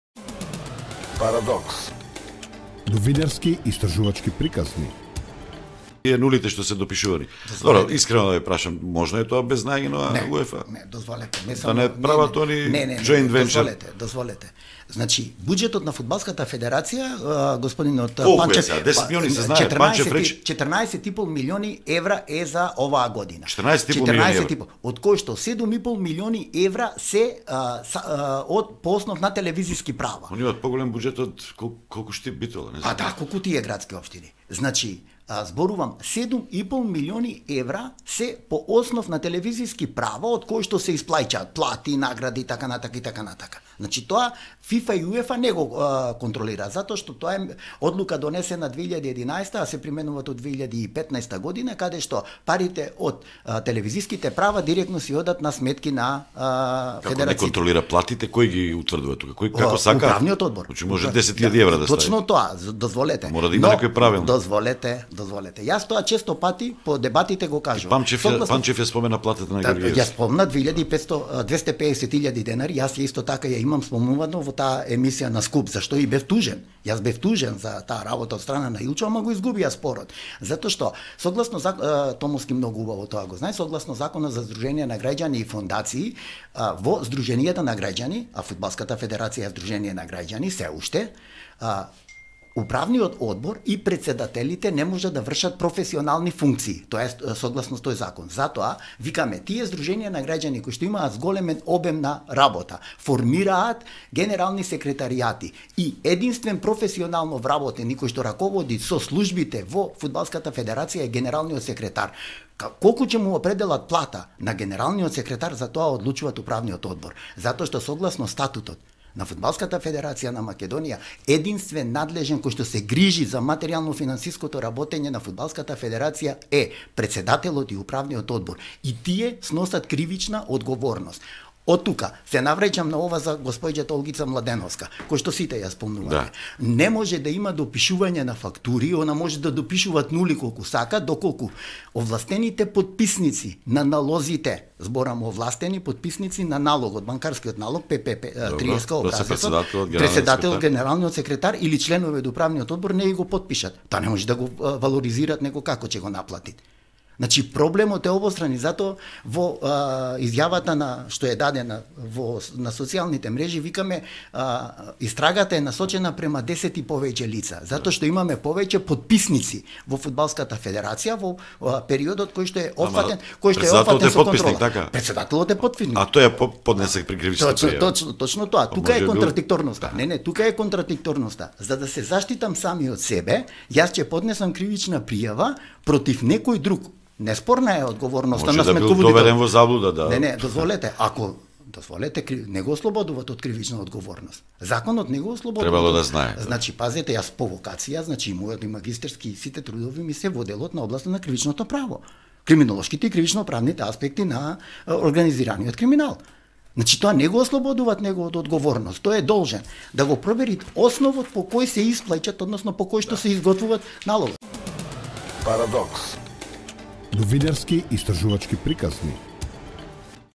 Љубомир Ѓурчевски во Стадион
Буџетот на ФФМ е 14 милиони евра, ако крадела службеничката која додавала нули тоа сигурно го знаеле и треба да одговараат и претседателот и генералниот секретар